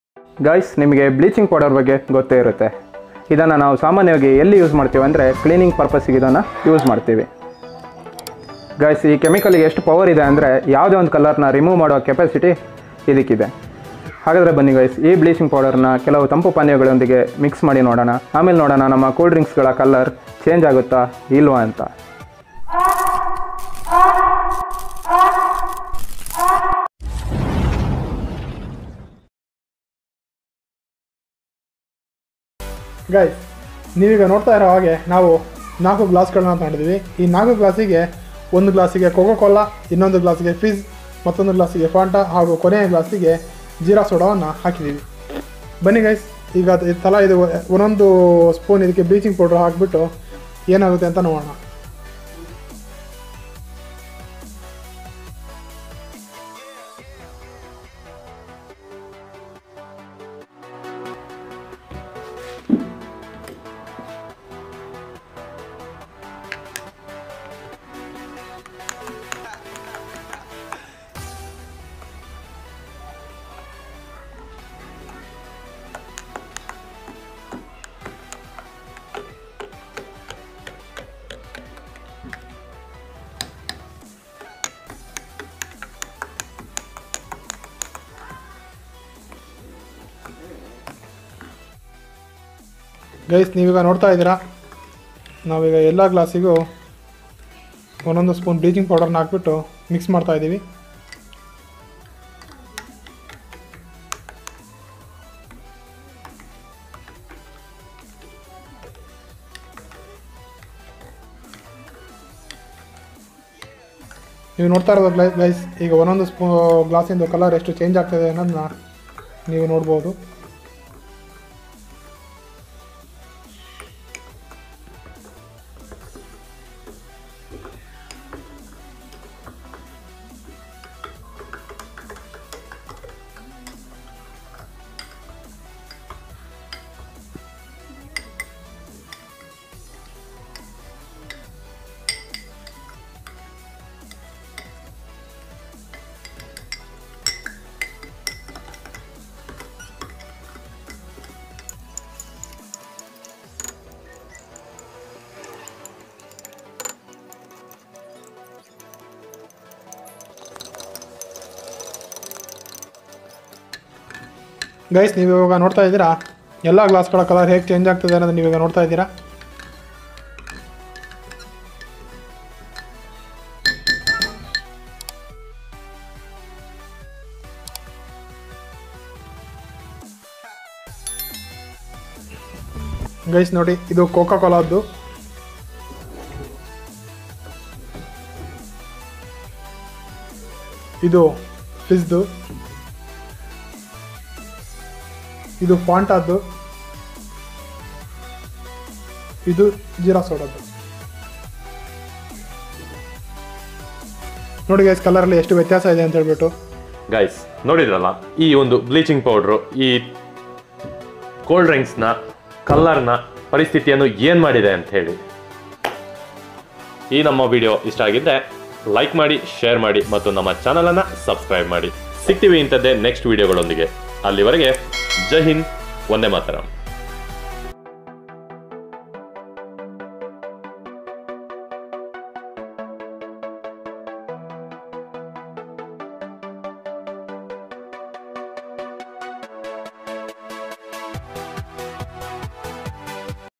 BleachVsCold drinks,Bleaching All Cold Drinksಎಲ್ಲಾ ಕೋಲ್ಡ್ ಡ್ರಿಂಕ್ಸ್ ಕಲರ್ ಲೆಸ್ ಆಗಬಹುದಾ,Power Hackers
What happens if we mix bleaching powder with cold drinks???
Bleaching Coca-Cola,Fanta,Fizz and Jeera.

Give a thumbs-up(Like) and a Share.